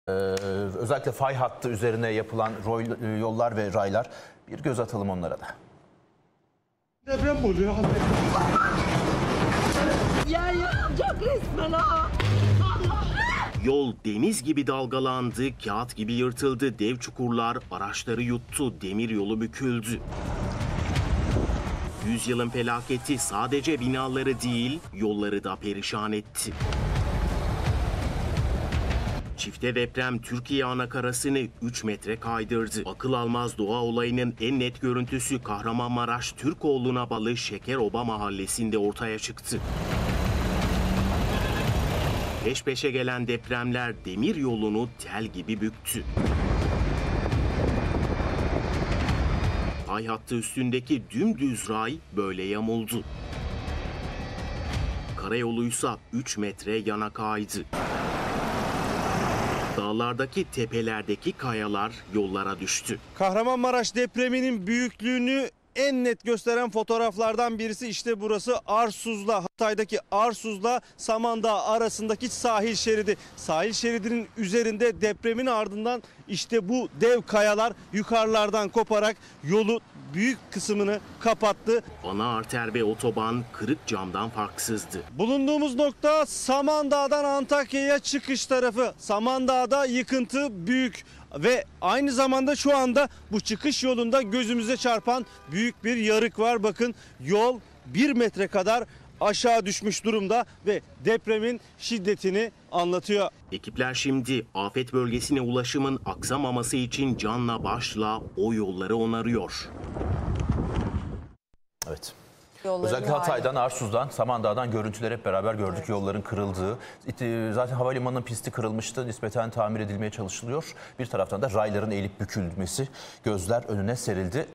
Çifte deprem Türkiye'yi 3 metre kaydırdı
Kahramanmaraş merkezli 2 büyük deprem Türkiye'yi 3 metre kaydırdı. Fay hattının kırılması sonucu kayan yer kabuğu yıkıma sebep oldu